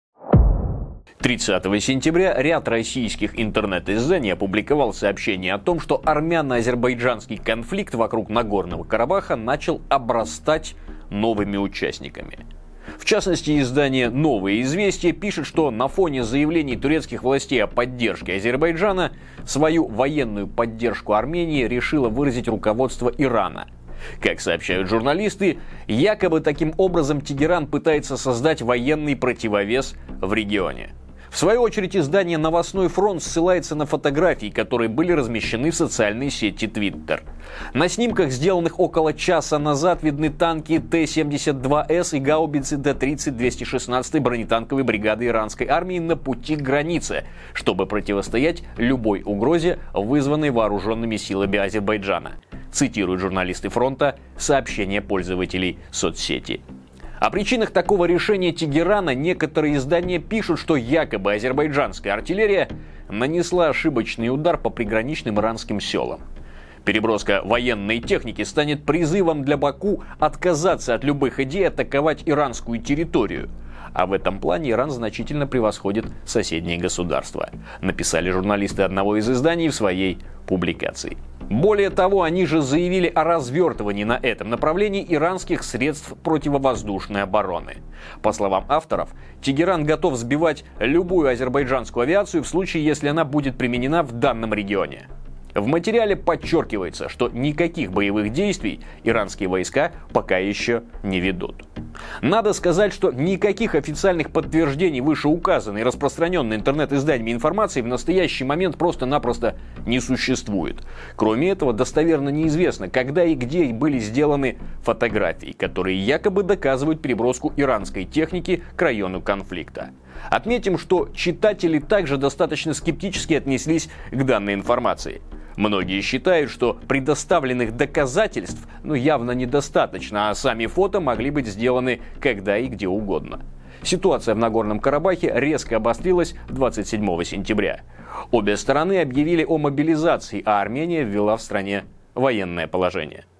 Иран перебрасывает танки в поддержку Армении
30 сентября ряд российских интернет-изданий опубликовал сообщение о том, что армяно-азербайджанский конфликт вокруг Нагорного Карабаха начал «обрастать» новыми участниками.